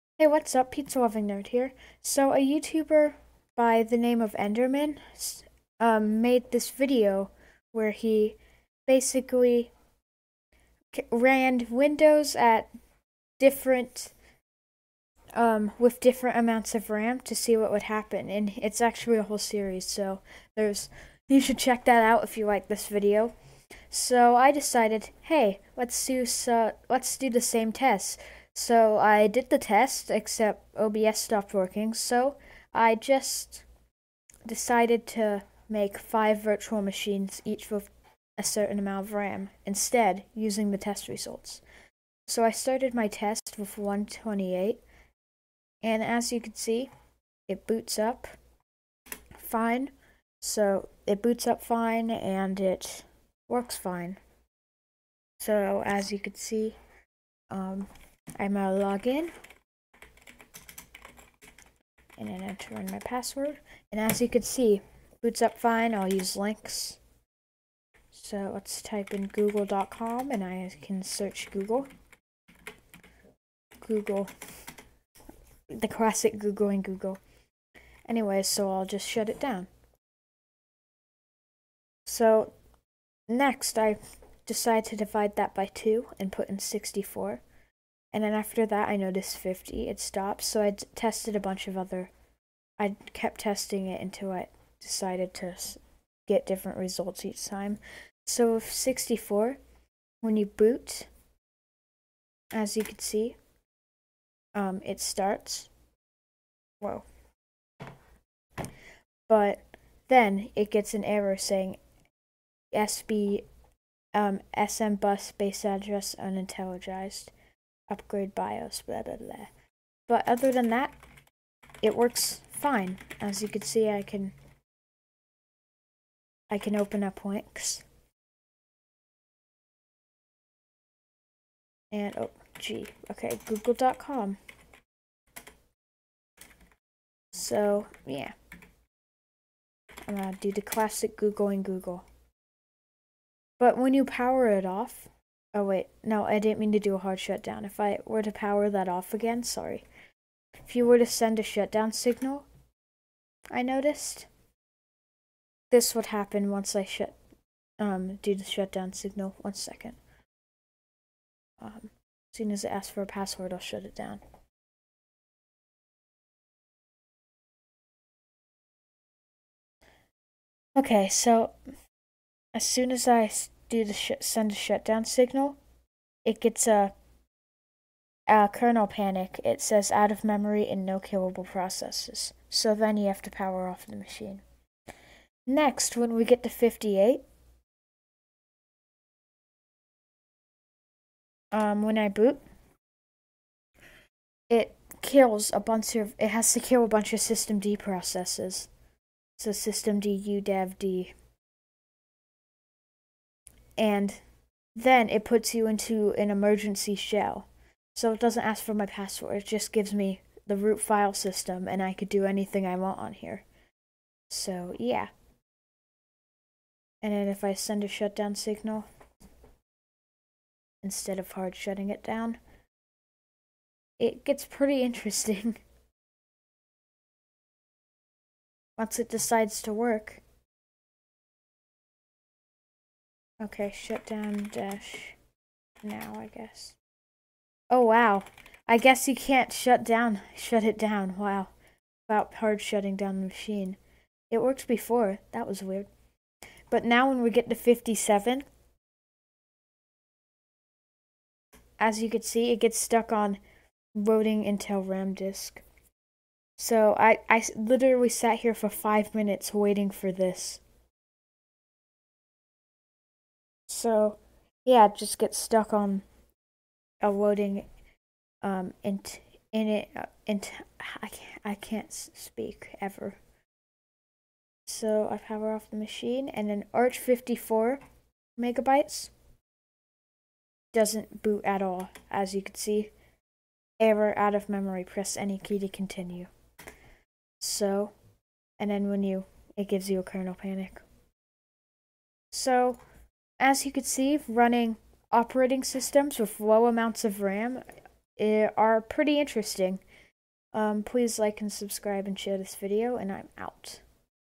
How Much RAM Do You Need To Run Arch Linux
I decided to test Arch Linux using a VM.

128 Megabytes 0:42
64 Megabytes 1:30
58 Megabytes 3:03
57 Megabytes 4:05
54 Megabytes 4:36